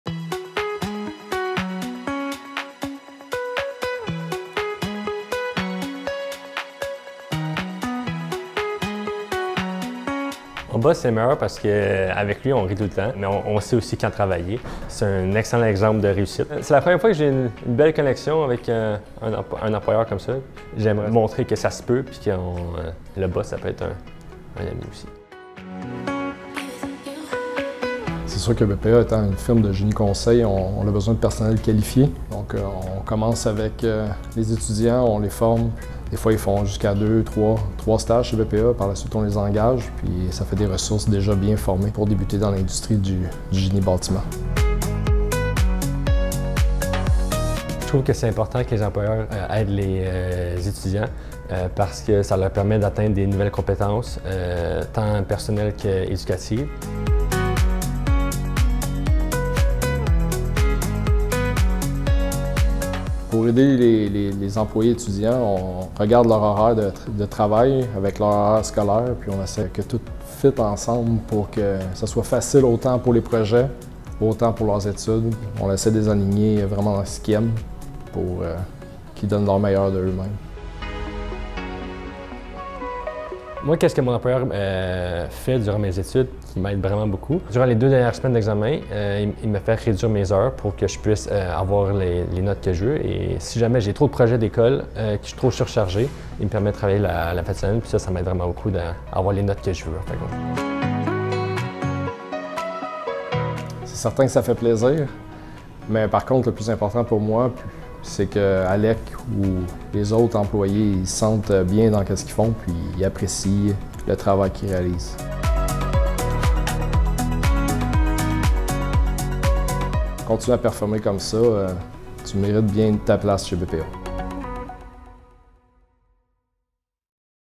Mon boss, c'est le meilleur! Laval 2023 - DUO #4 Guillaume Bouccara de BPA et Alek Lanoie
Guillaume Bouccara est directeur de Puissance et infrastructures critiques /électricité à Bouthillette Parizeau, une firme de génie-conseil à Laval. Son employé Alek Lanoie, un étudiant du Collège Montmorency, a soumis son nom au concours 𝐌𝐨𝐧 𝐛𝐨𝐬𝐬, 𝐜'𝐞𝐬𝐭 𝐥𝐞 𝐦𝐞𝐢𝐥𝐥𝐞𝐮𝐫! parce qu'il favorise sa réussite éducative, notamment par des pratiques qui facilitent la conciliation études-travail.
Tous les deux font partie des six duos gagnants de la première édition lavalloise du concours!